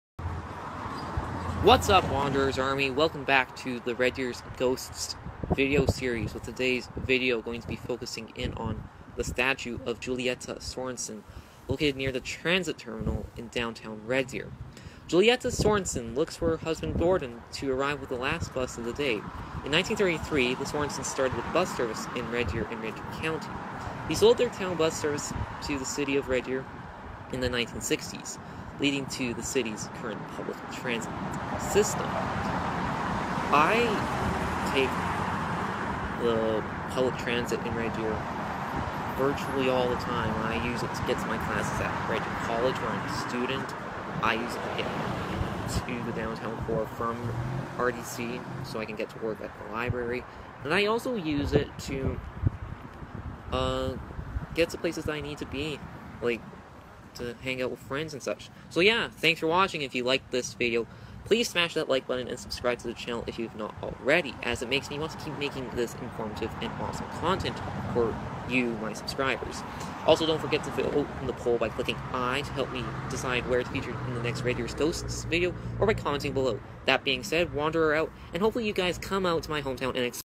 Red Deer's "Ghosts"- Waiting for Gordon | theWanderers-notebook
What's up Wanderers! Welcome back to the Red Deer’s “Ghosts” video series with today’s video going to be focusing in on the statue of of Julietta Sorensen located near the transit terminal in downtown Red Deer. Julietta Sorensen looks for her husband, Gordon, to arrive with the last bus of the day. In 1933, the Sorensens started a bus service in Red Deer and Red Deer County. They sold their town bus service to ¬ the City of Red Deer in the 1960s, leading to the city’s current public transit system.